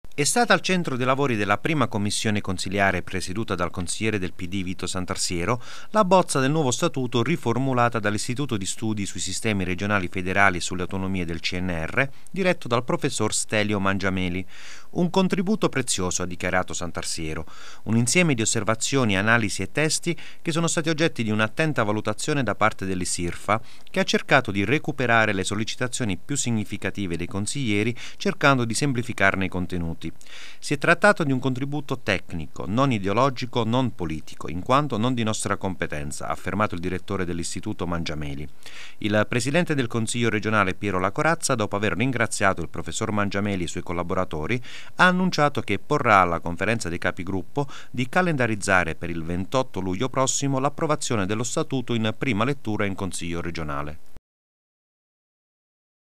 STATUTO REGIONE, IN I CCP CONSEGNATA PROPOSTA ISSIRFA
Mangiameli: " Un contributo tecnico, non politico e ideologico"